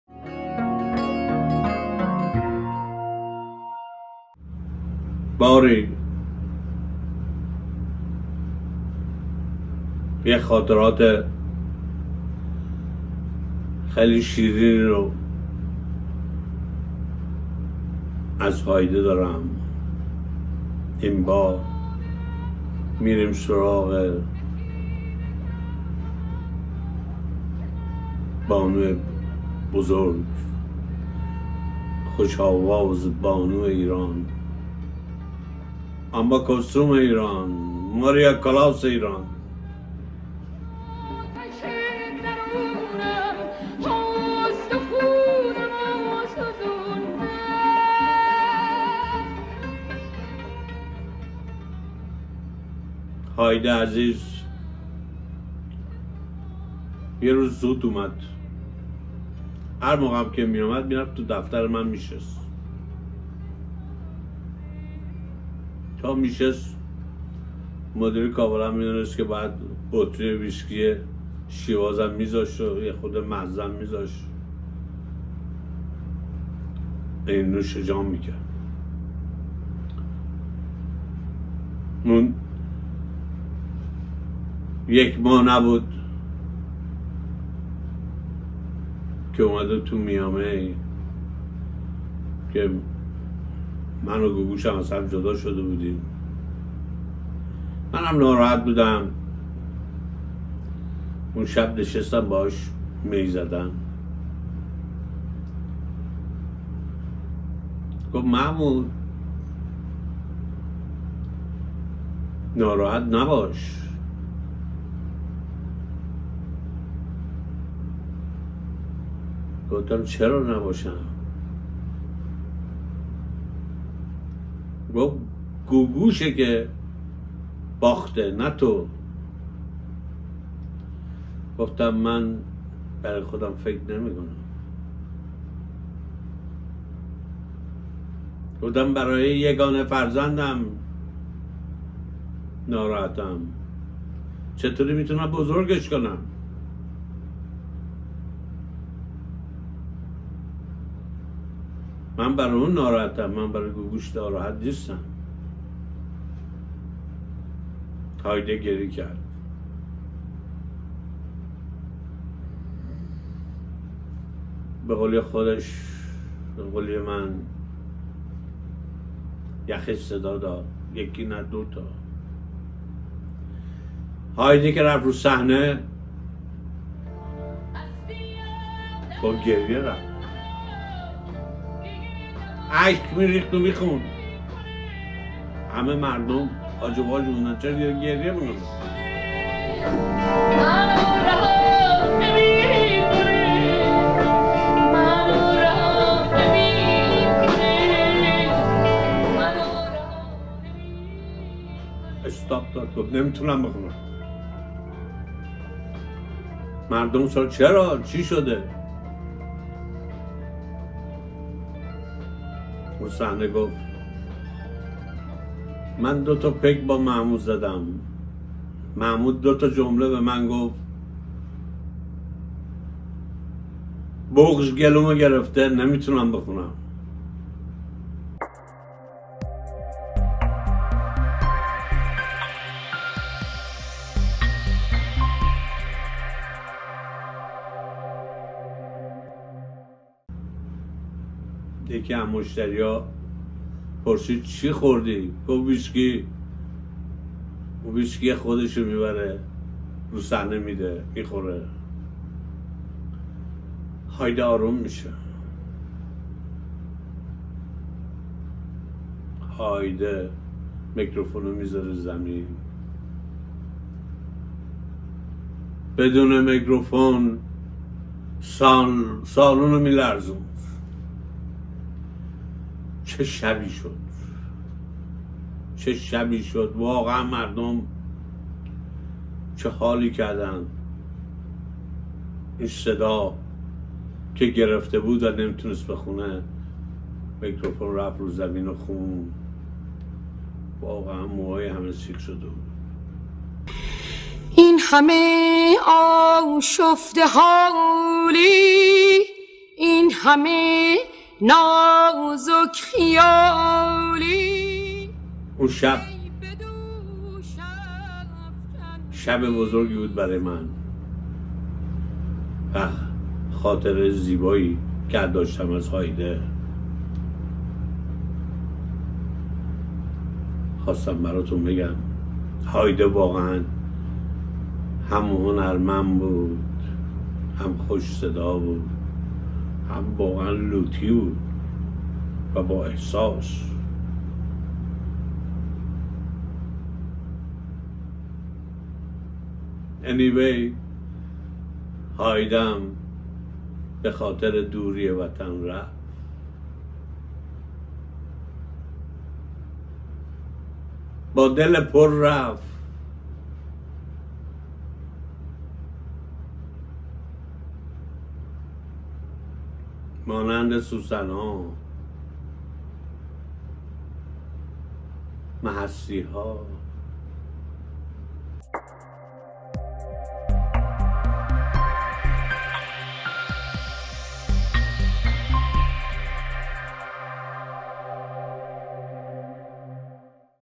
‎ زندگینامه محمود قربانی (قسمت ۲۸۲ ) شبی که هایده با گریه خوند
‏‎‏‎خلاصه ای از زندگینامه و خاطرات محمود قربانی در سالیان عمر هنری و کارنامه کاری ايشان